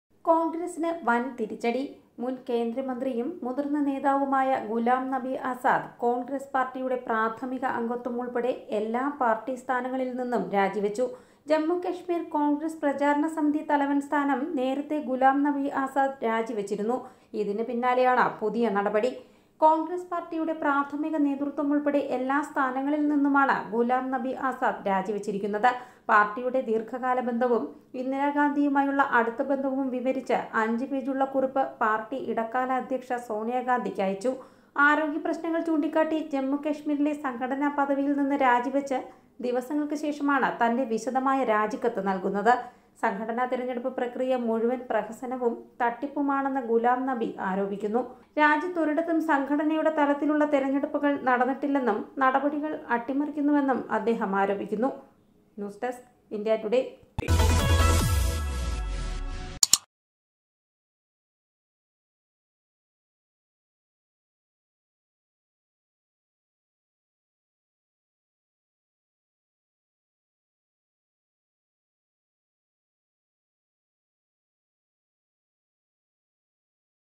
ഗുലാം നബി ആസാദ് കോണ്‍ഗ്രസില്‍ നിന്ന് രാജി വച്ചു | Ghulam Nabi Azad resigned from Congress
മുന്‍ കേന്ദ്രമന്ത്രിയും മുതിര്‍ന്ന നേതാവുമായ ഗുലാം നബി ആസാദ് കോണ്‍ഗ്രസ് പാര്‍ട്ടിയുടെ പ്രാഥമിക അംഗത്വം ഉള്‍പ്പെടെ എല്ലാ പാര്‍ട്ടി സ്ഥാനങ്ങളില്‍ നിന്നും രാജിവച്ചു.